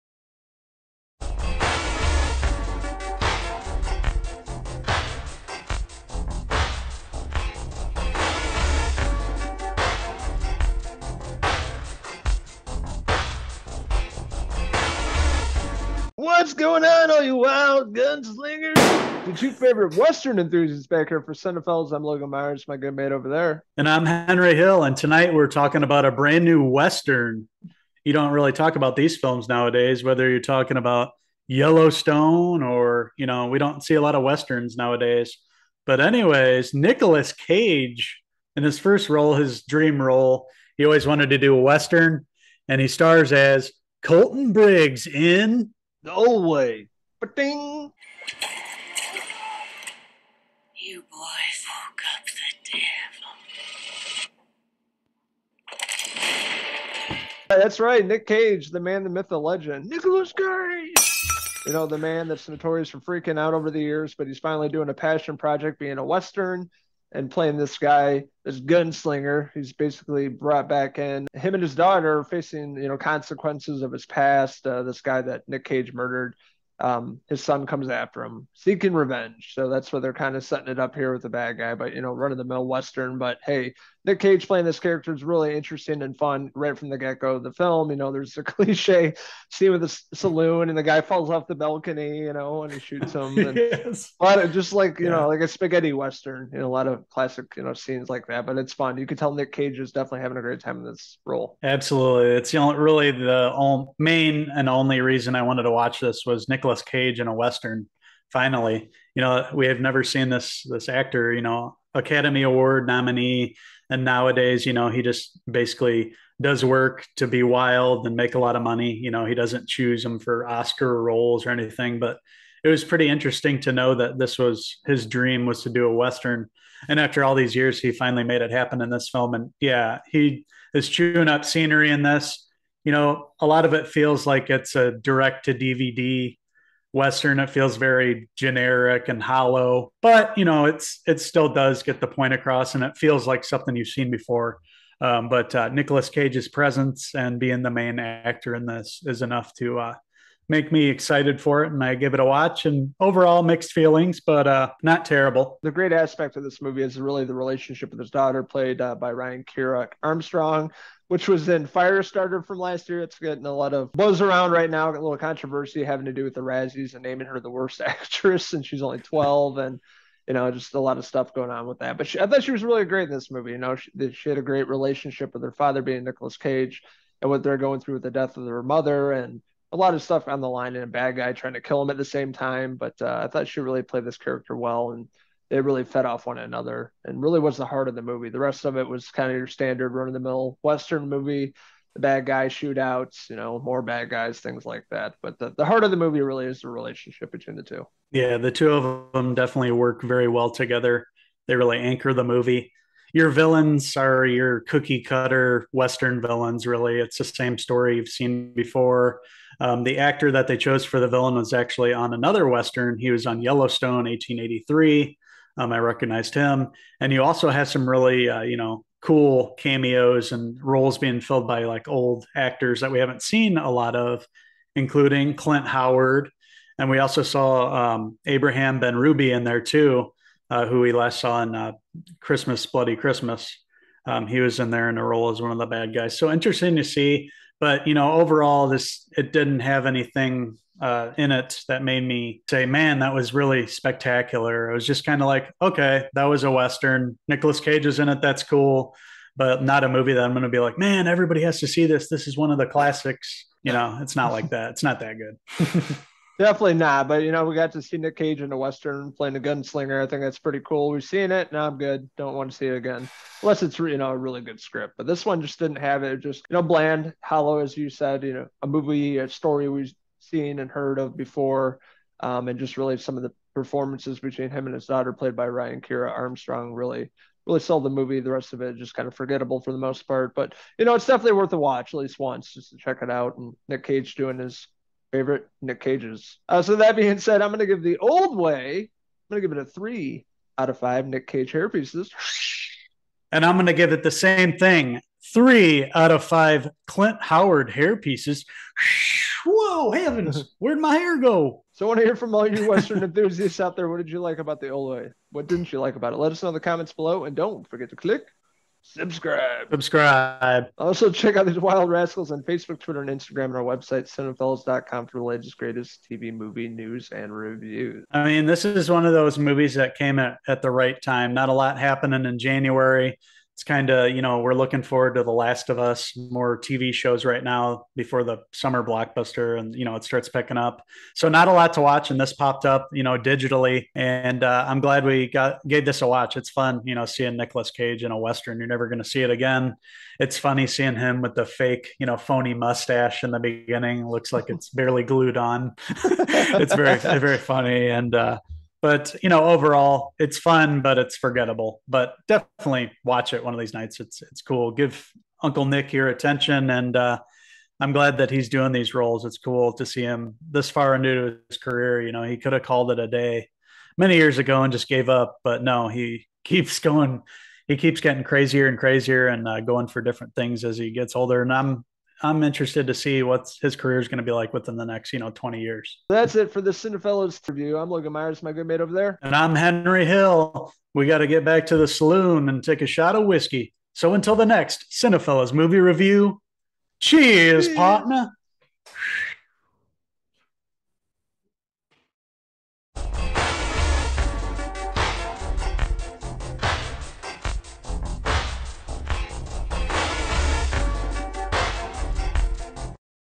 The Old Way - Movie Review
An old gunslinger (Nicolas Cage) and his daughter must face the consequences of his past, when the son of a man he murdered years ago arrives to take his revenge in ‘The Old Way’. Pull up stool at the saloon and enjoy our review of the movie, now available on digital to rent or own.

"An old gunslinger and his daughter must face the consequences of his past, when the son of a man he murdered years ago arrives to take his revenge."